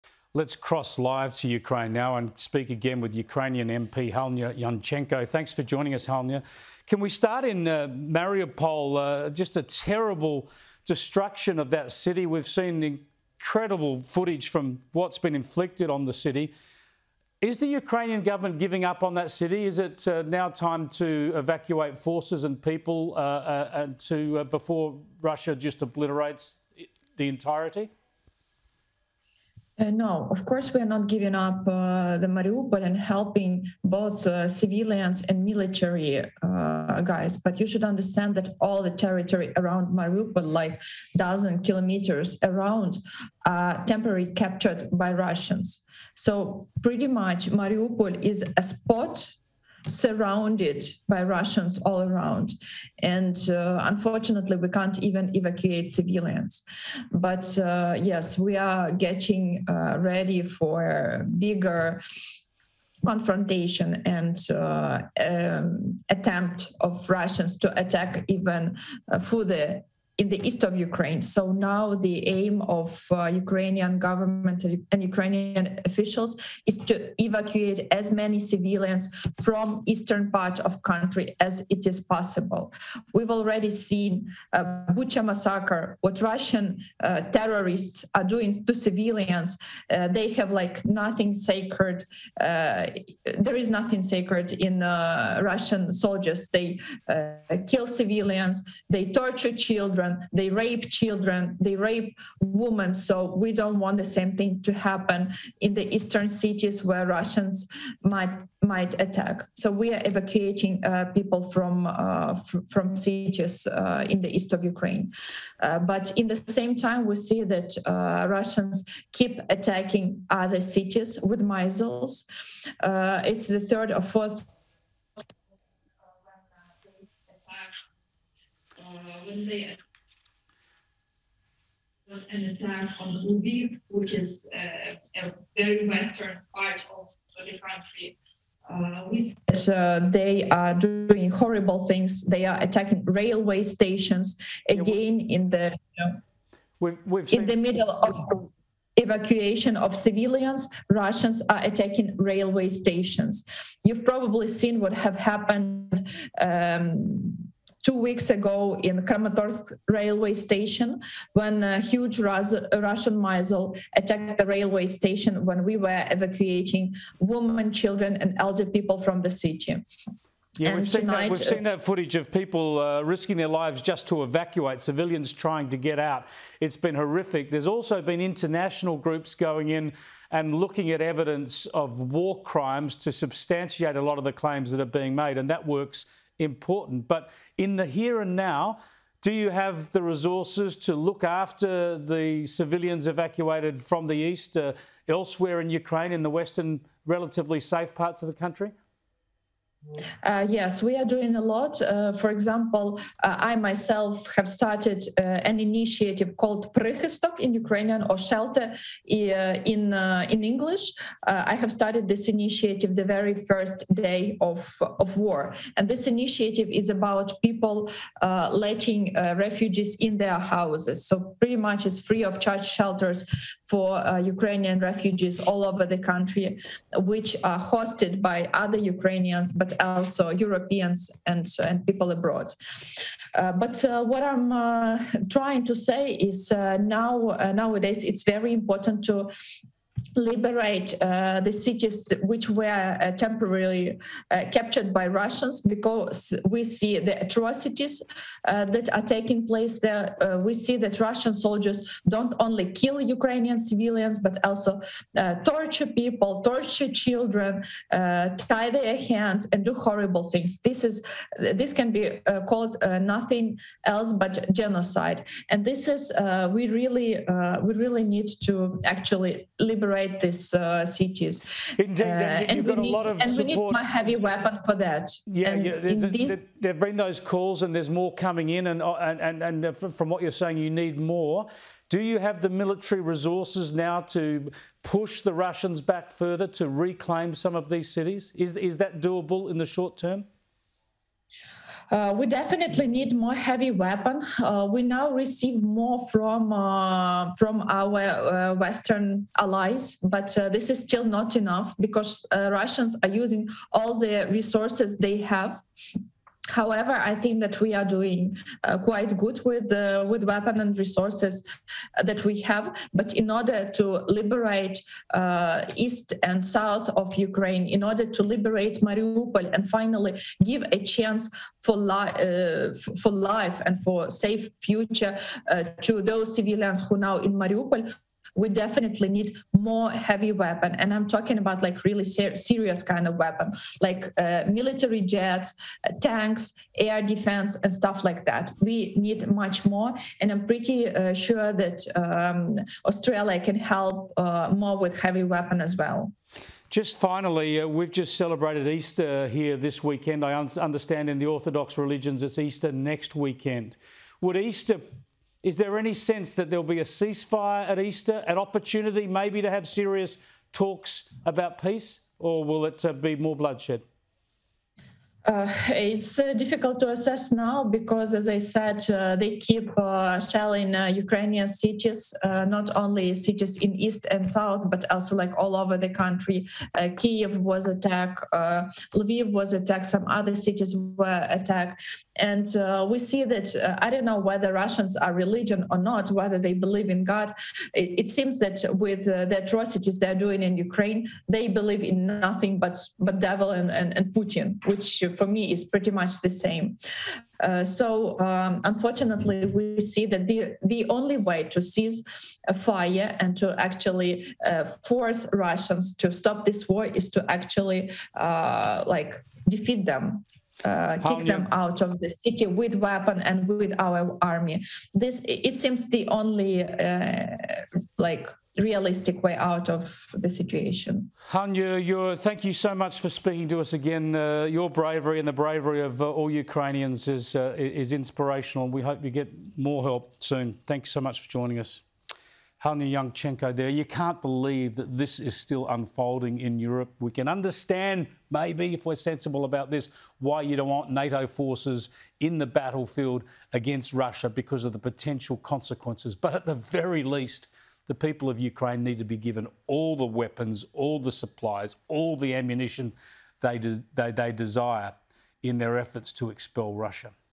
Ukrainians 'not giving up' on Mariupol but need 'more heavy weapons'
Ukrainian MP Halyna Yanchenko is calling for more heavy weapons from the country's "western allies" as they try to hold off Russian forces from capturing the city of Mariupol.

Ms Yanchenko said Ukrainian officials are still committed to helping “both civilians and the military” in the region.

"Now the aim of Ukrainian government officials is to evacuate as many civilians from the Eastern part of the country as possible," Ms Yanchenko told Sky News host Chris Kenny.

The Ukrainian MP said the country is still “doing quite good” with the weapons and resources they have been provided with but need more to “liberate the east and south of Ukraine".

“In order to liberate Mariupol and finally give a chance for life and a safe future to those civilians now in Mariupol, we definitely need more heavy weapons.”